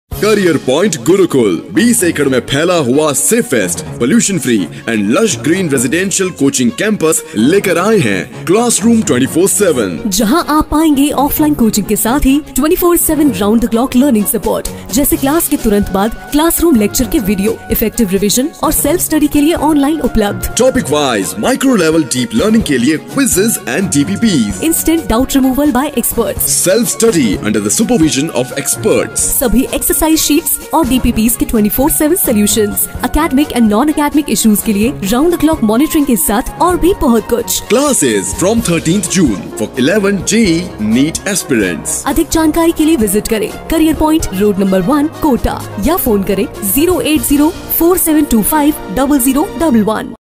𝐂𝐋𝐀𝐒𝐒𝐑𝐎𝐎𝐌 𝟐𝟒/𝟕 𝐑𝐞𝐬𝐢𝐝𝐞𝐧𝐭𝐢𝐚𝐥 𝐂𝐨𝐚𝐜𝐡𝐢𝐧𝐠 With Round The Clock Support | Career Point Gurukul
Career Point Gurukul announces Classroom 24/7 Residential Coaching With Round The Clock Support for JEE & NEET aspirants.

Coaching + Hostel + School in 20 Acres lush green campus in Kota having all facilities within campus.

◾️ Time Saving
◾️ Cost Effective
◾️ Round the Clock Learning Support
◾️ Safe & Secure Campus
◾️ Motivating Environment
◾️ Life Skill Development